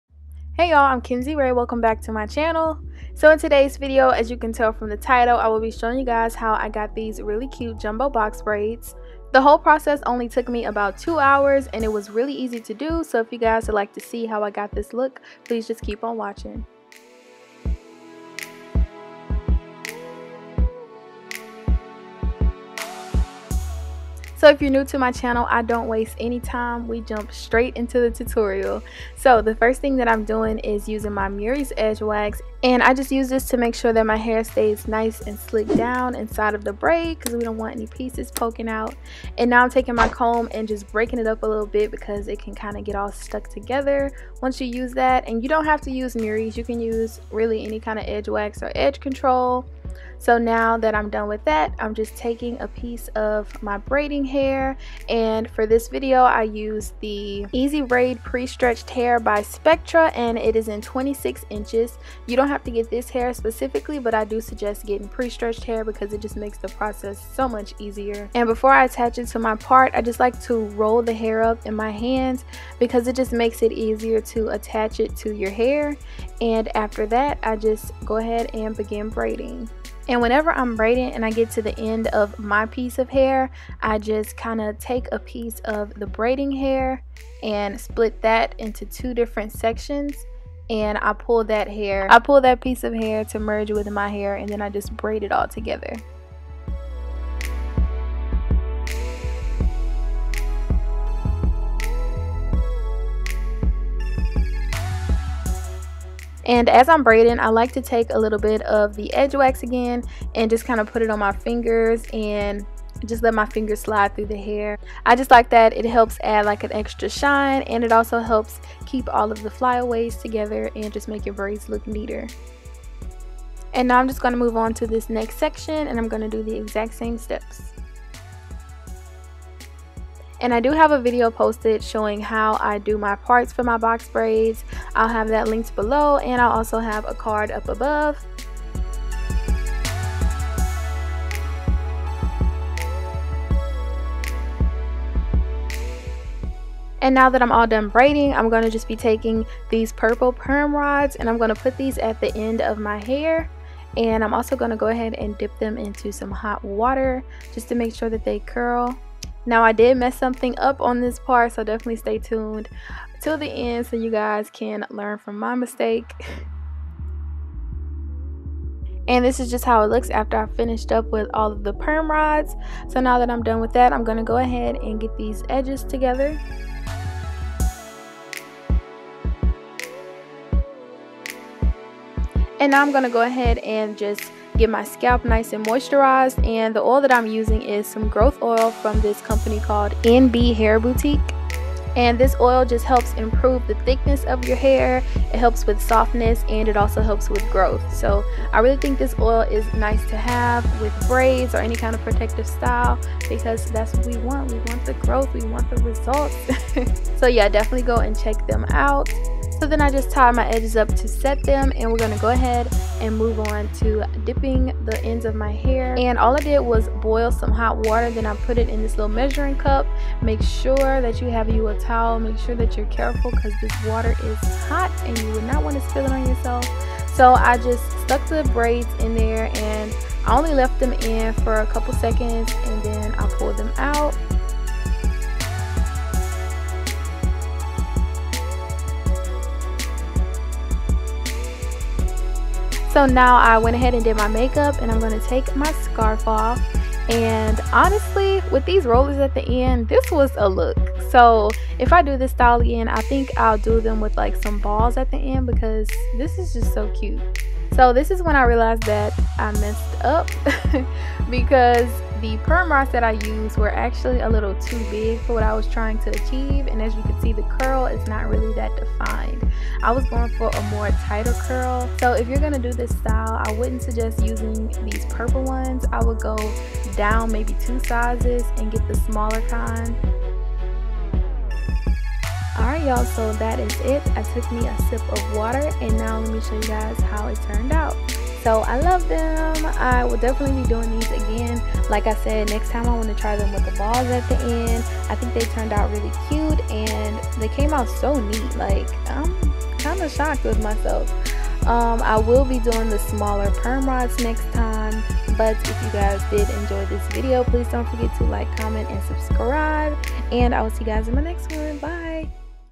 EASY Jumbo Box Braids + Rubber-band Method | Beginner Friendly | Protective Style
Hey friends! In today's video I'll be showing y'all how I did these really easy Jumbo Box Braids! Box braids are definitely one of favorite protective styles. Mainly because they're easy to install, require very little maintenance, and last up to 3 months with proper care! For this style I used 5 packs of Spectra EZ Braid 1B in 26 inches (linked below) and it took me about 2 hours to complete.

Products used in this video:
Growth Oil:

Murrays Edge Wax:

**HELP ME GET TO 100,000 SUBBIES **

***ABOUT ME***
Location: Louisiana